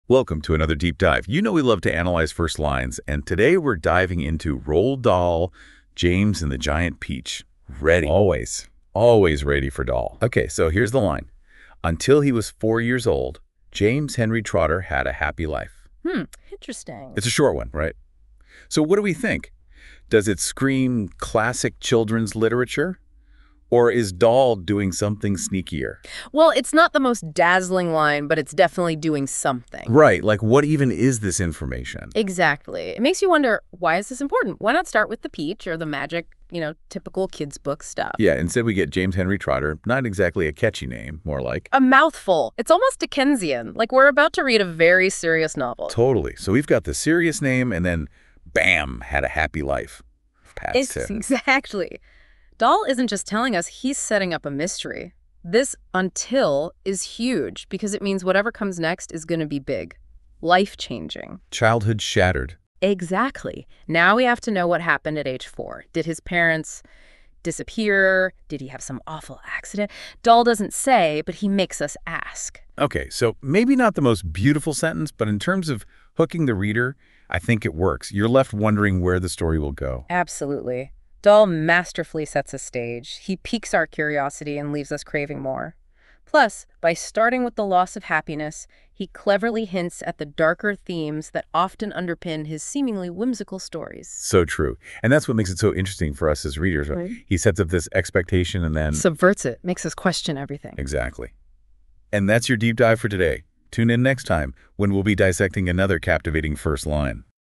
Hooked from the First Line - James and the Giant Peach
This excerpt from James and the Giant Peach by Roald Dahl gives us a glimpse into the early life of James Henry Trotter, the protagonist of the story. The passage describes his happy childhood up to the age of four, providing the foundation for the fantastical adventures that await. As the story progresses, James's life will change in unimaginable ways, leading to a journey full of wonder, magic, and excitement.